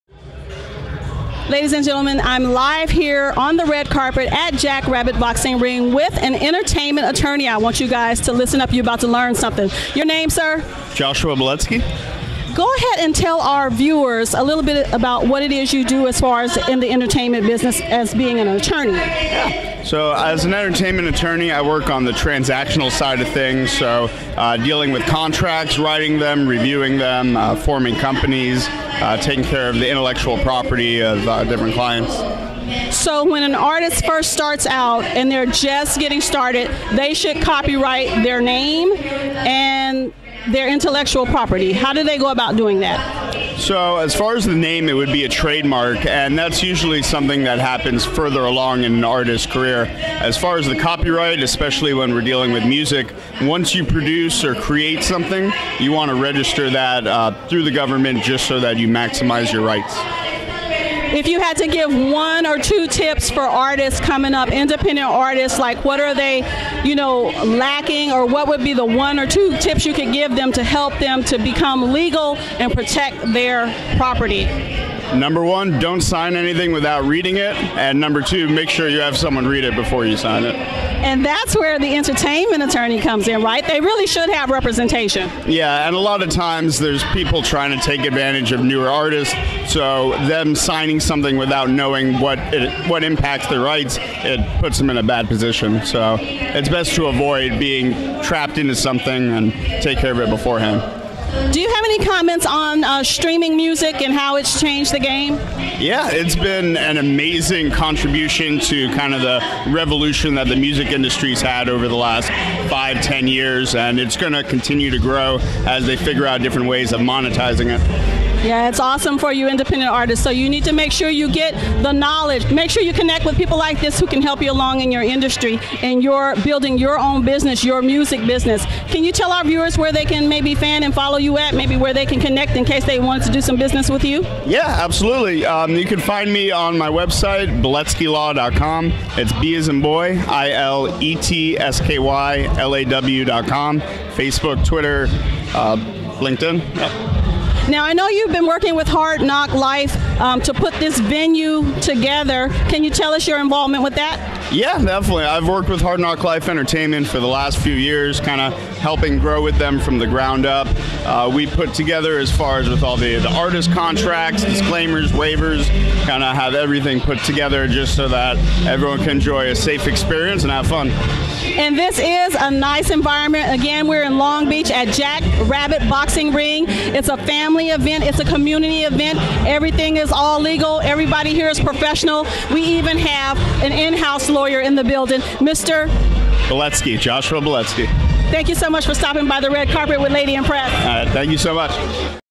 Lashaun Turner aka Lady I.M.Pre$S talks with entertainment Attorney Joshua M. Biletsky
Lashaun Turner aka Lady I.M.Pre$S talks with entertainment Attorney Joshua M. Biletsky on the Red Carpet at "The Process" event in Long Bech California. Cali's BEST Radio Station